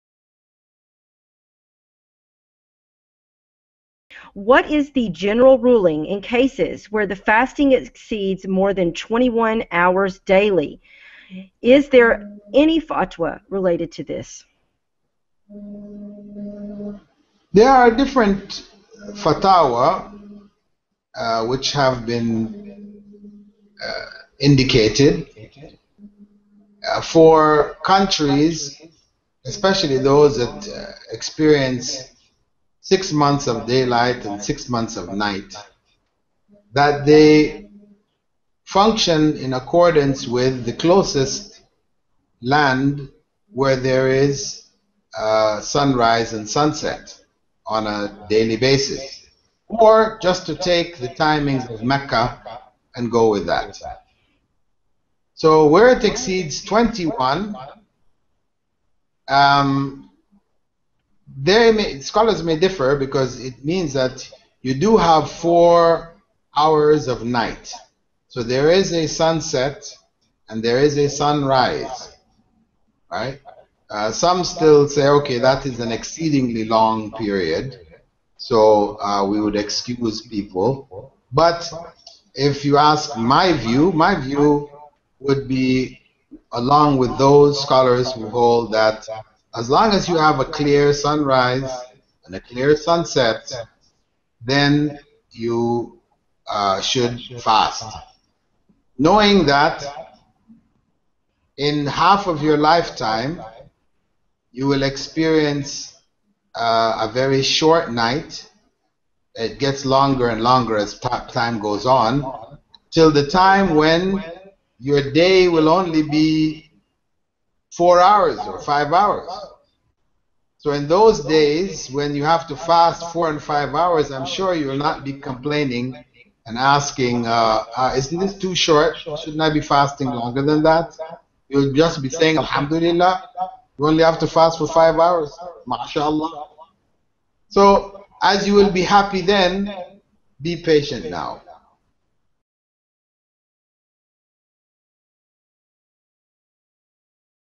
QA - What is the ruling if the fast exceeds more than 21 hours?
Selected QA from the webinar conducted by the Islamic Online University

"Ramadan Made Easy - Dr. Bilal Philips"

Become a seeker of Beneficial Knowledge
JOIN the Islamic Online University NOW!!
Access authentic Islamic Knowledge ONLINE!!